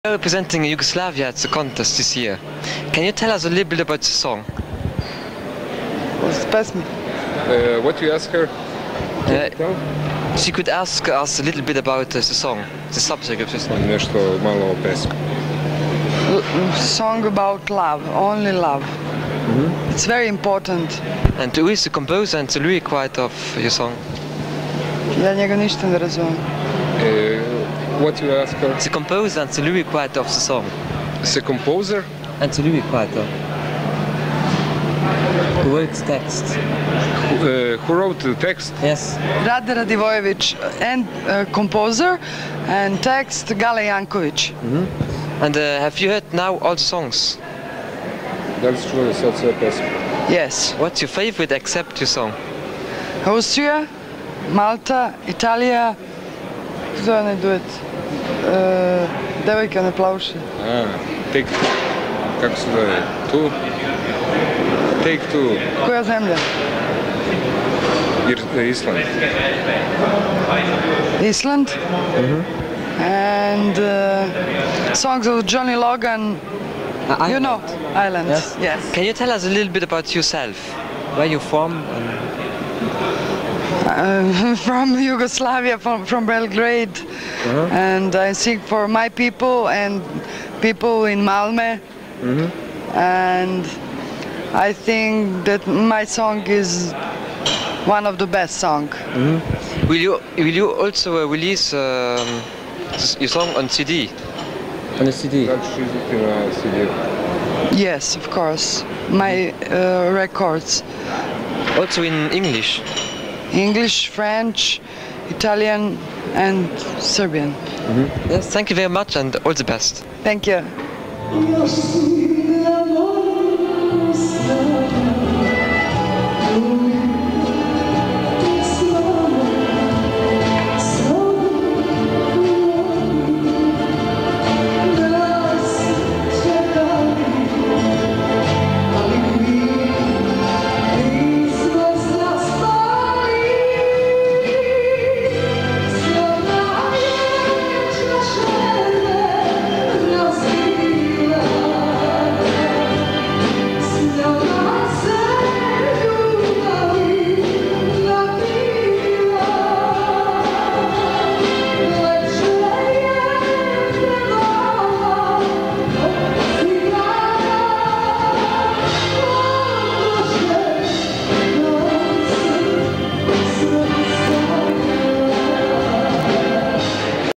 Extra Nena - Evrovizija 1992
Yugoslavia's rehearsal on Eurovision song contest in 1992. Original upload by @peterhenshaw8678